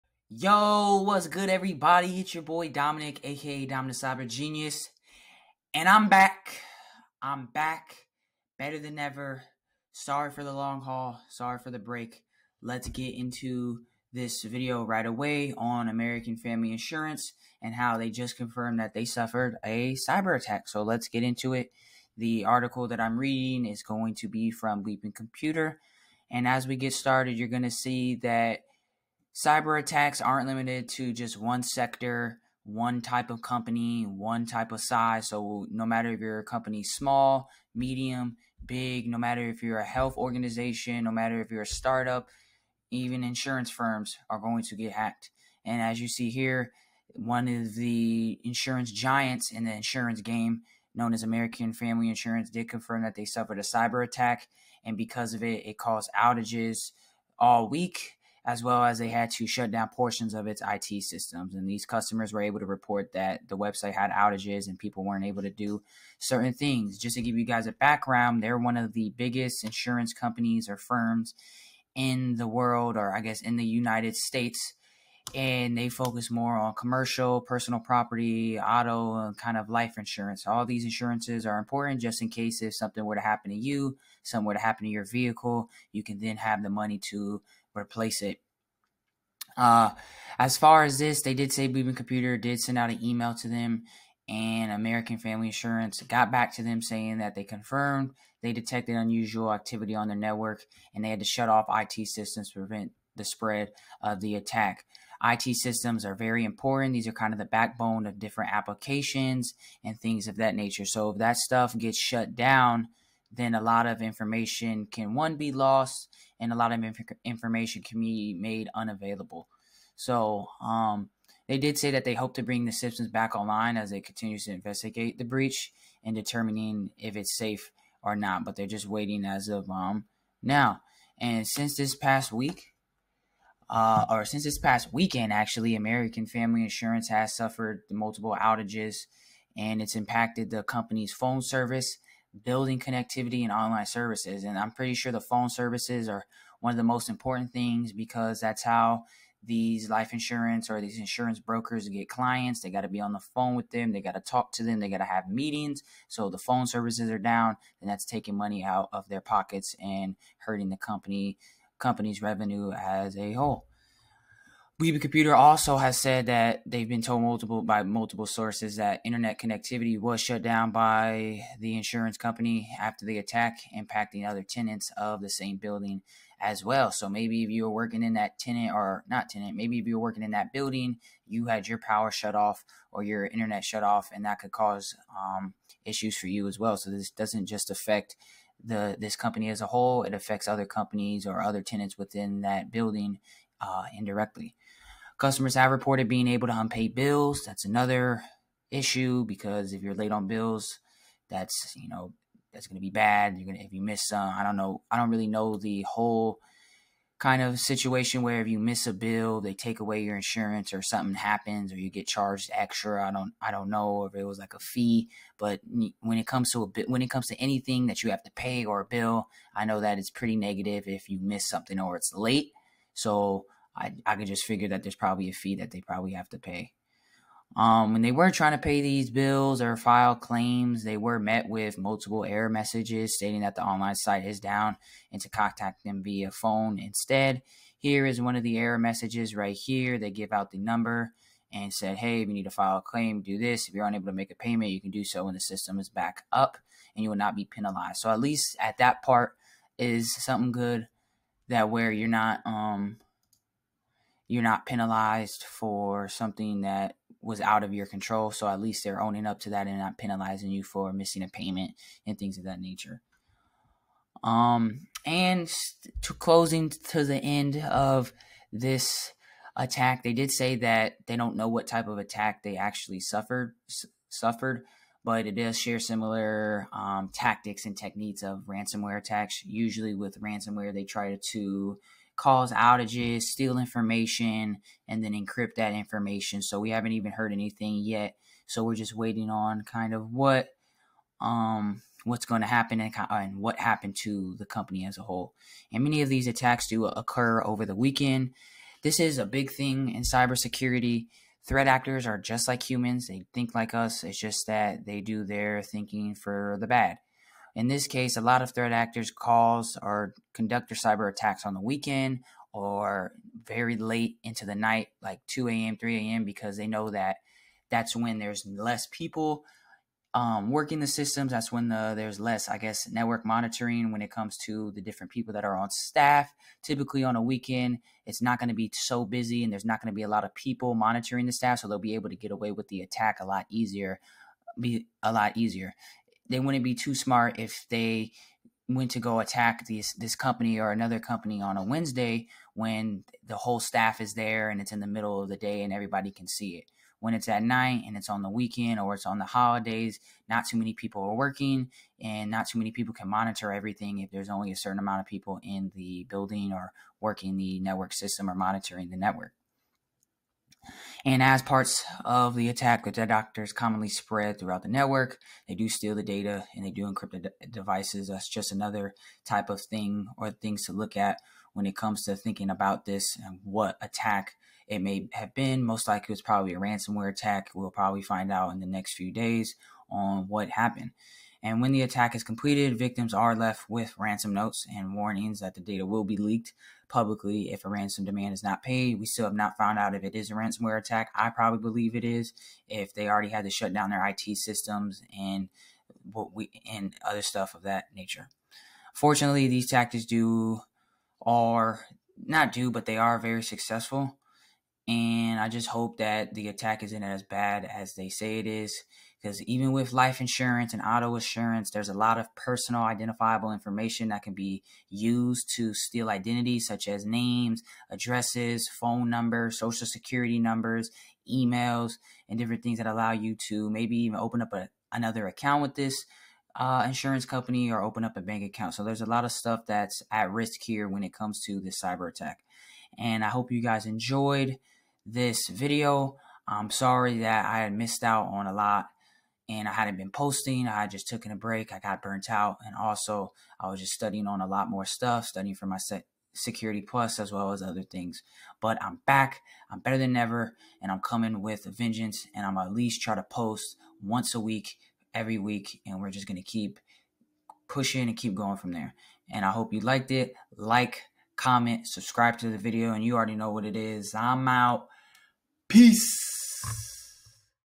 Inside the Hack: The American Family Insurance Breach Revealed!
Hello YT family IM BACK! Here is a video on American Family Insurance suffering a cyberattack.